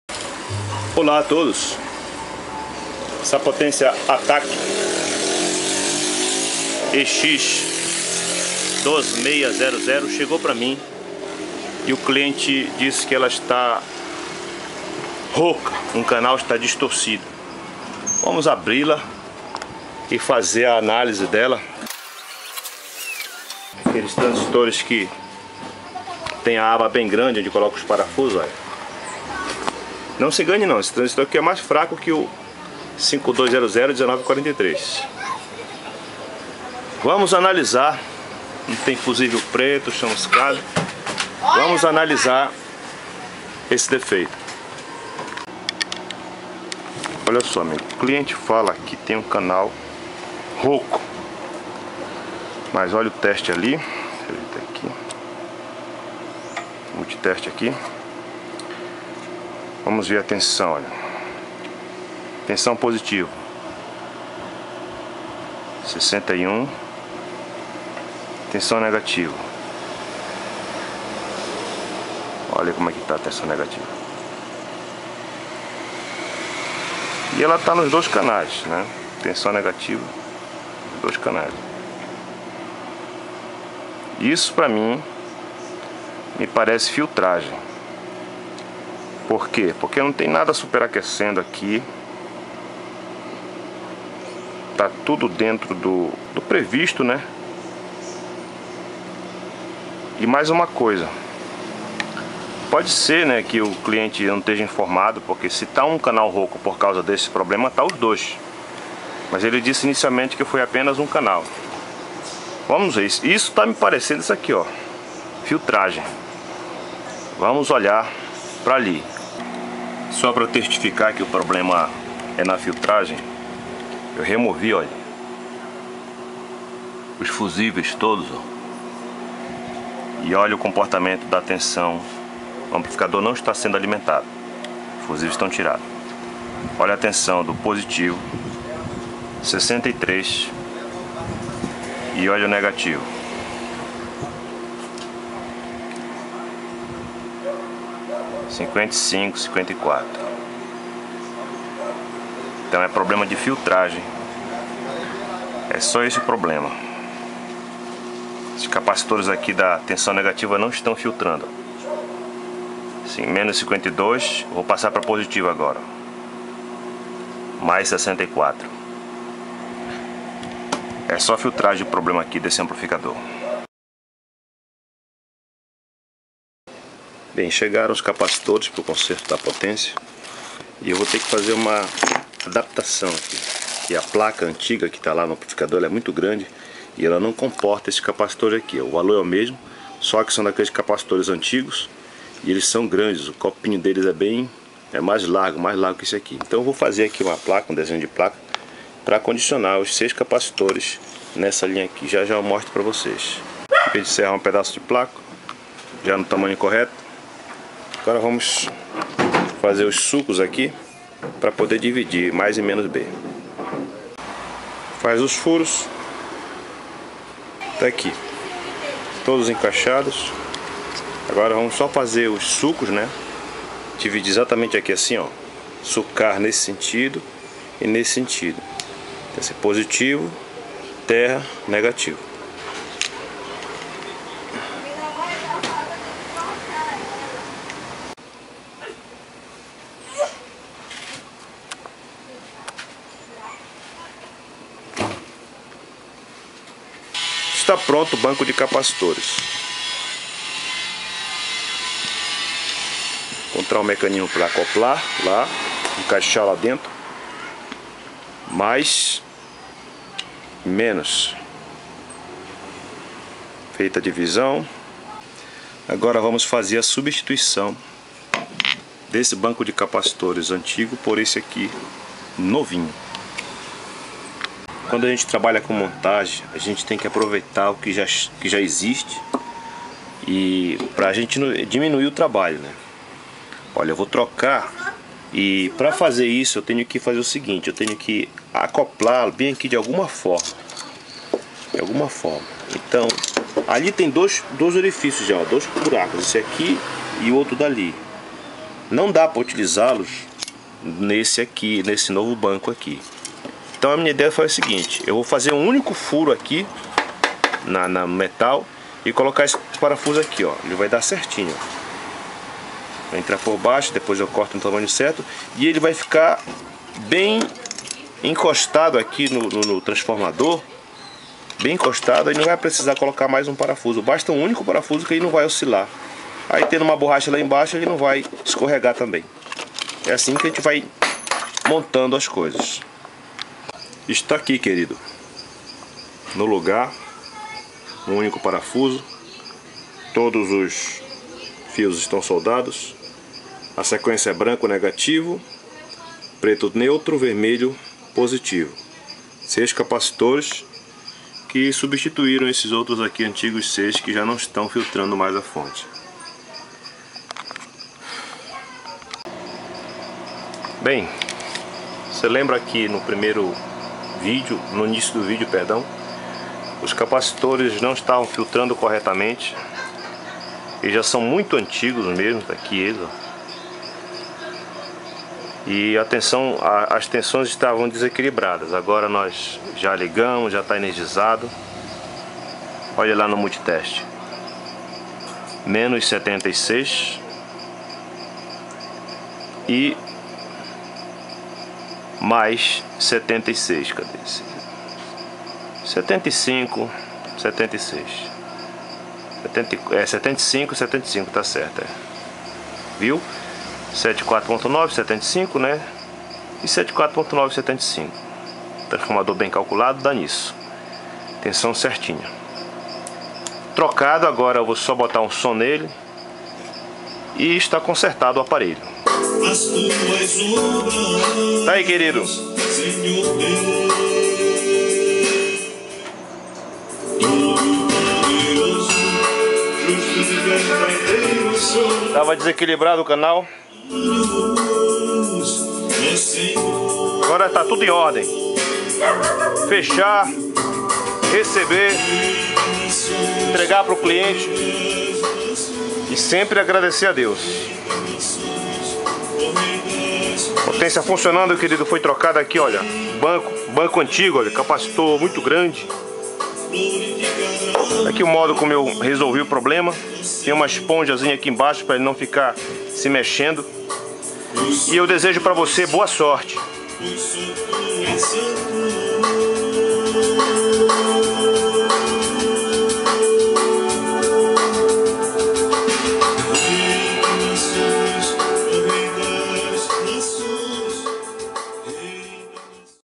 Defeito e conserto Amplificador ATTACK EX 2600 turbo
Essa potência já foi "o máximo" e ainda agora ela responde muito bem quando tudo está em ordem. Com a filtragem baixa o aparelho distorcia após a identificação do defeito e troca dos componentes o aparelho funcionou normalmente.